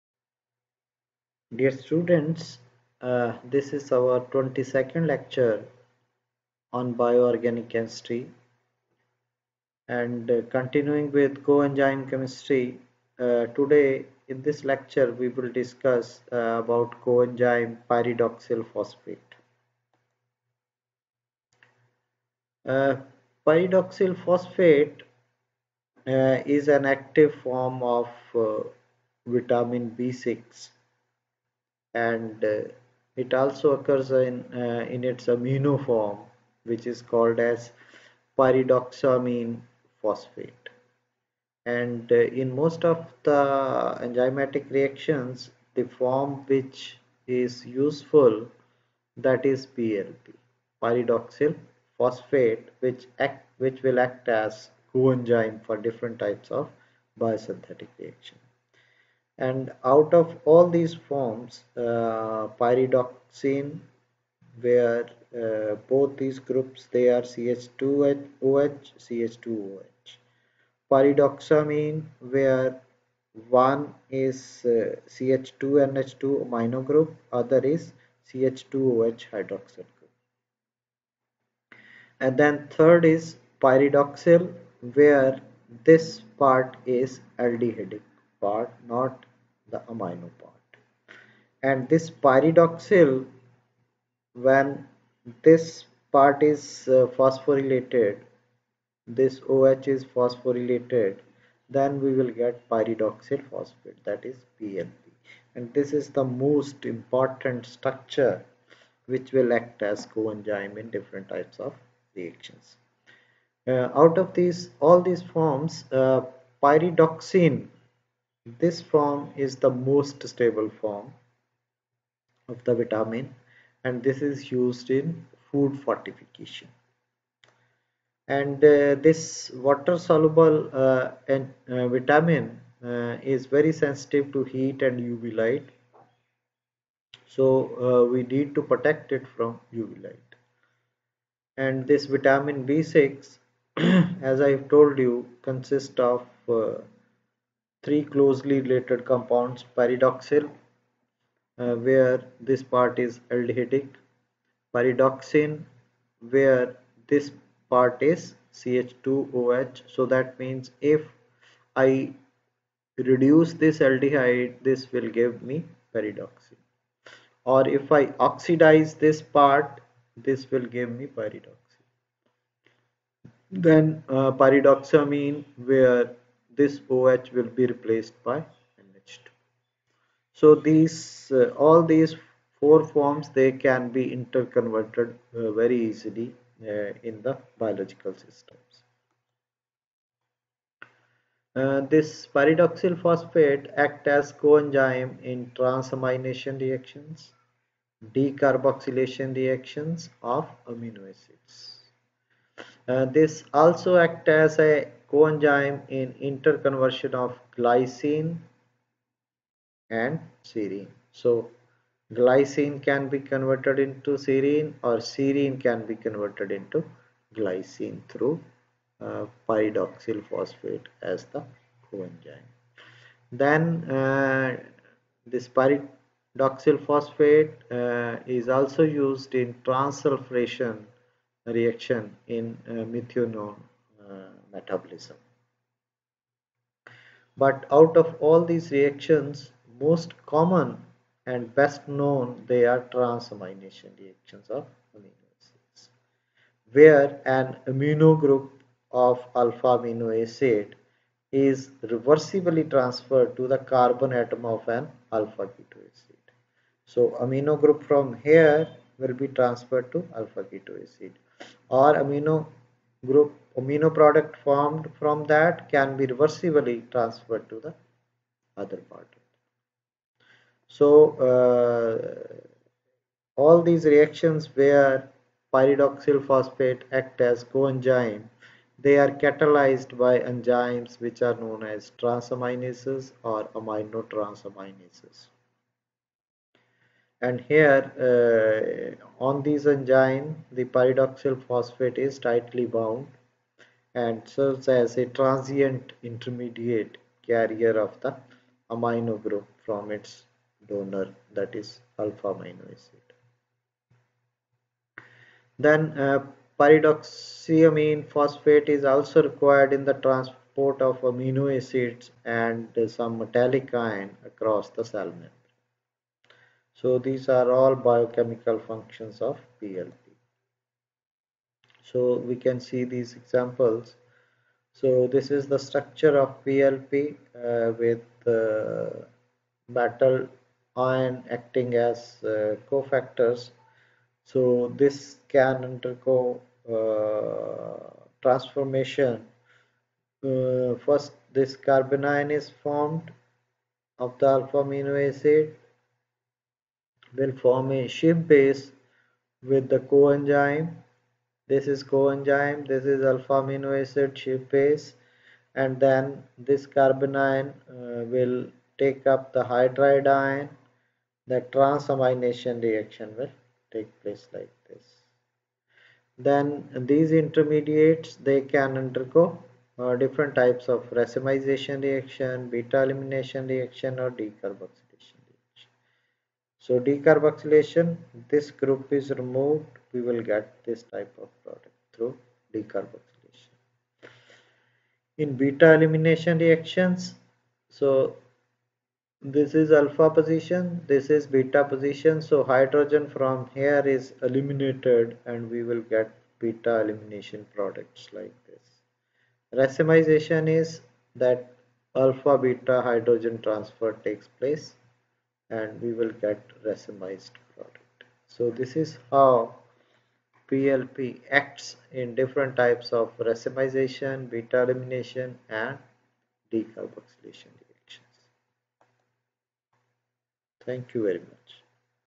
Coenzyme Chemistry (Pyridoxal Phosphate) | Prepare for GATE, JAM, NET
Learn Coenzyme Chemistry (Pyridoxal Phosphate) in detail with this online chemistry class. Preparation for CSIR-NET GATE IIT-JAM.